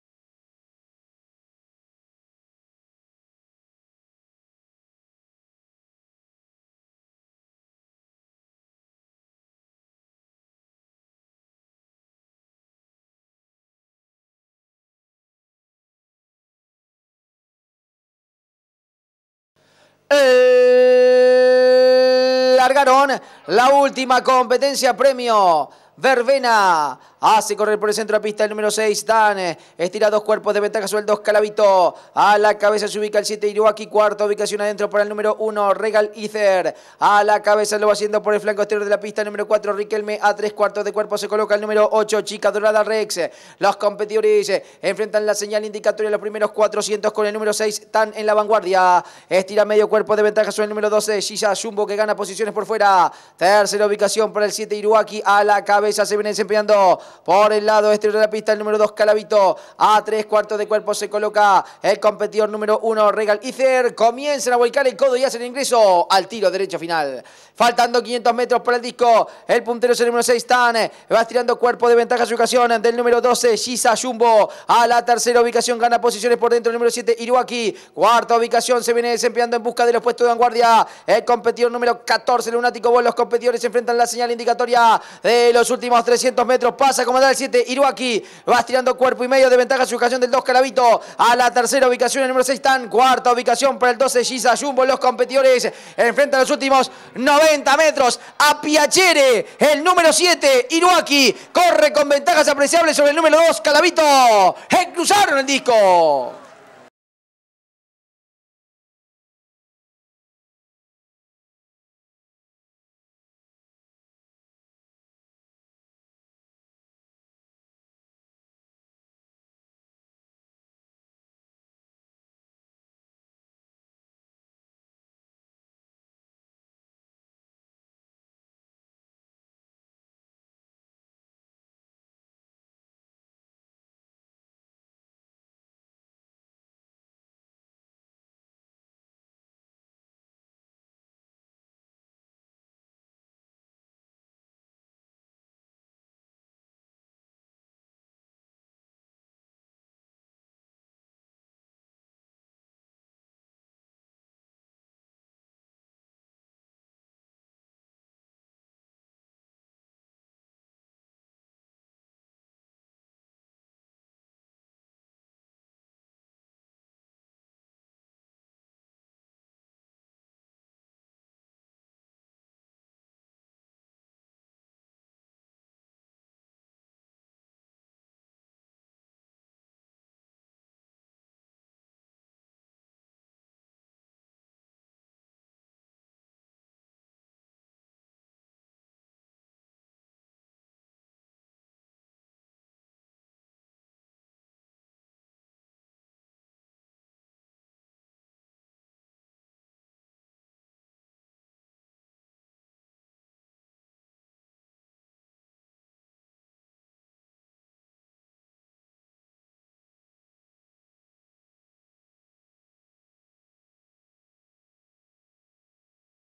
24/07/2016 – Hipodromo Colonia – Carr 7 – VERBENA
Carrera 7 realizada el dia 24/07/2016 por el premio VERBENA en el Hipodromo Colonia